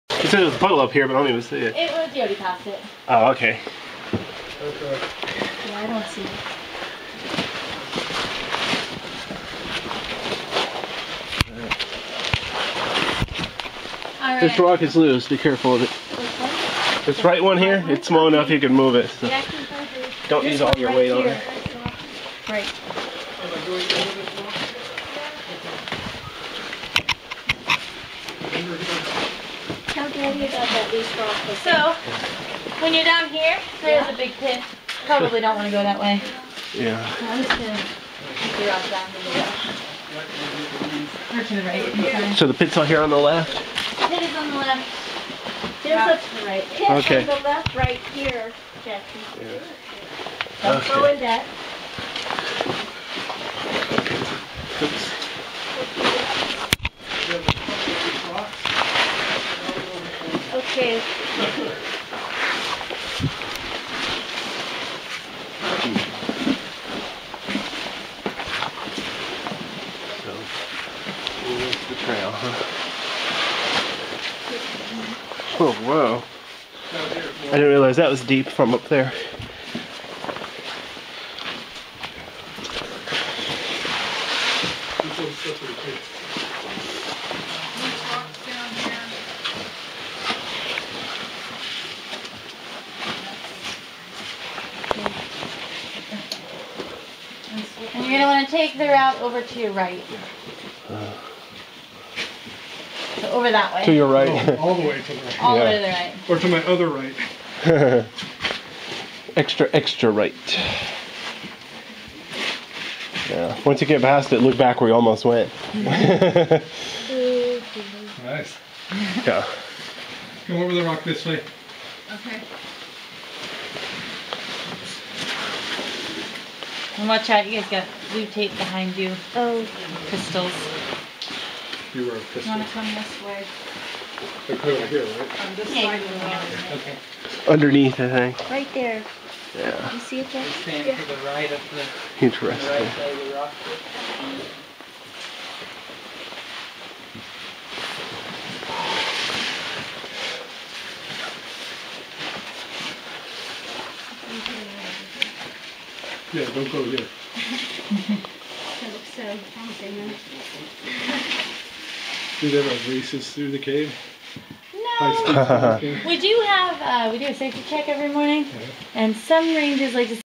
Oregon Caves, Wild Tour, part 3, HD
The camera and light didn't line up very well in this clip.This is only halfway throught he tour, but the battery in my camera died.  Better luck next time...